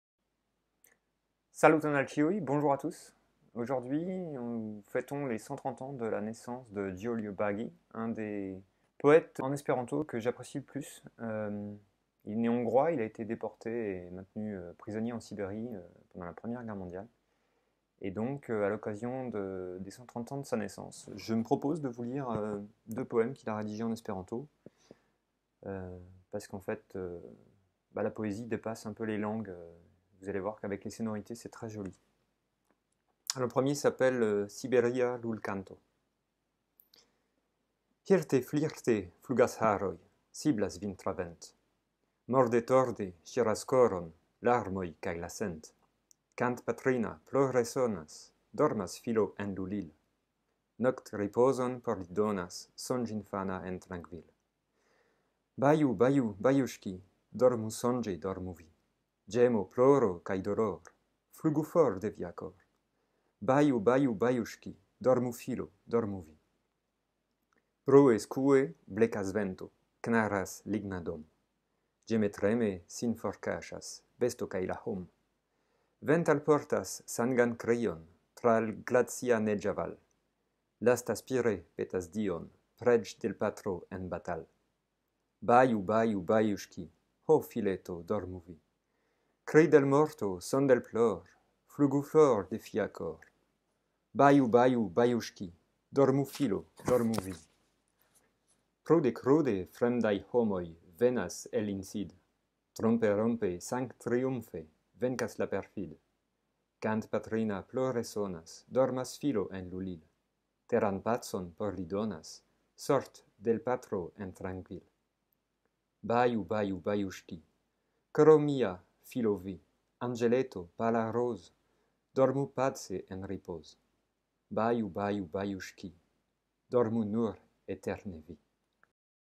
Aleks André legas poemon en esperanto de Julio Baghy
La franca samideano Aleks André legas poemon de la populara esperanta poeto Julio Baghy (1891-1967). Pliaj interesaj videoj de Aleks André en kaj pri esperanto videblas en lia kanalo: